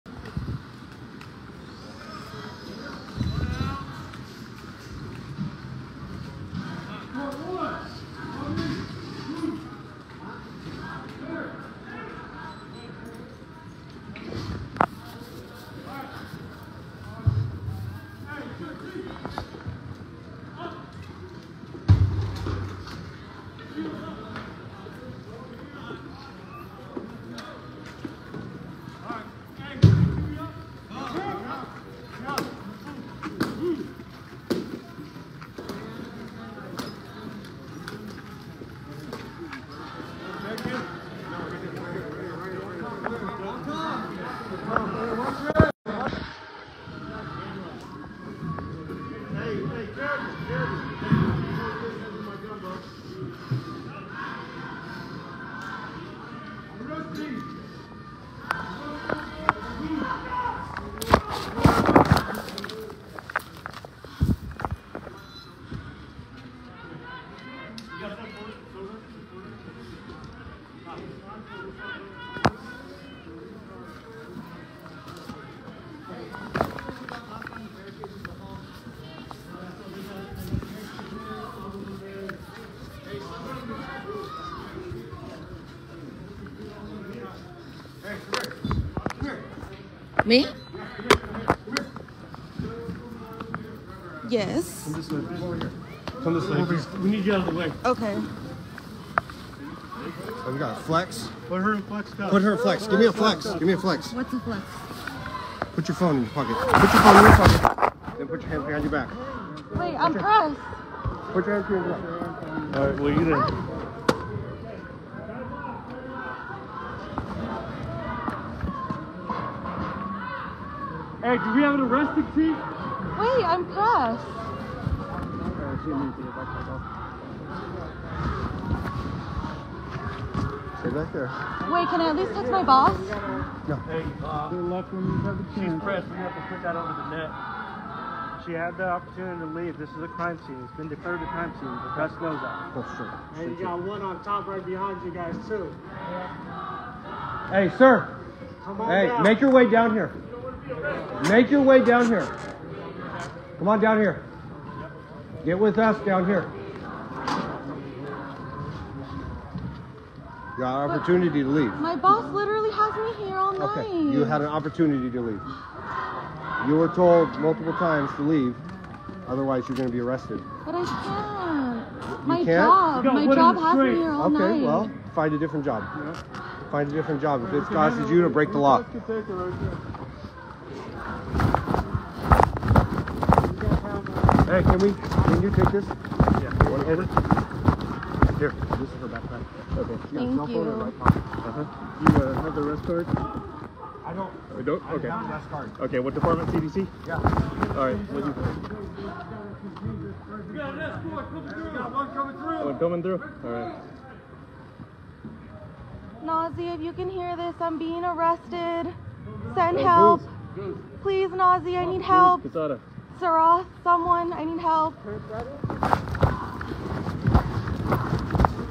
KRCR reporter detained while covering anti-war protests at California campus
Watch the moment KRCR/KAEF News Reporter Adelmi Ruiz is detained by law enforcement while covering a massive, multi-agency law enforcement sweep of Cal Poly Humboldt on Facebook Live early Tuesday morning, April 30, 2024. The sweep lead to over 30 arrests, including students and non-students, and ended the week-long occupation of Siemens Hall and Nelson Hall East.